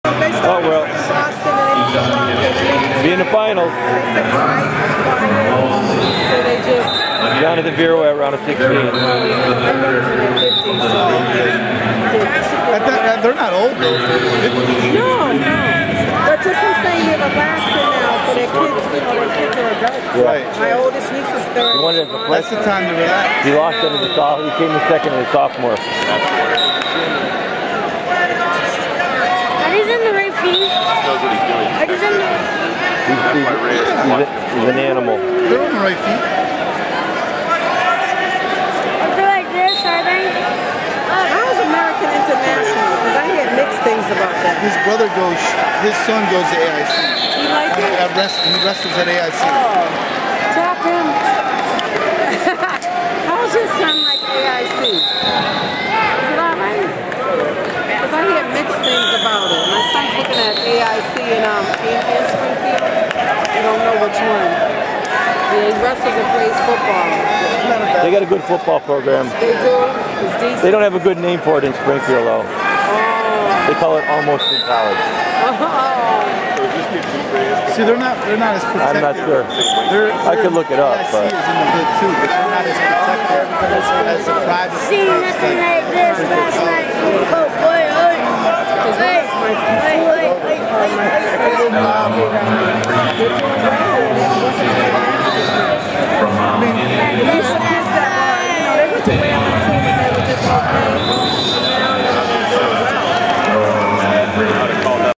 Jonathan Viruet (Central) vs AJ Grignaffini (Wellesley)
Jonathan Viruet (Central) vs AJ Grignaffini (Wellesley) in the 160 lb round of 16 at the Lowell Holiday tournament on December 27, 2014. The venue is the Tsongas Center. Viruet wins by fall at 1:22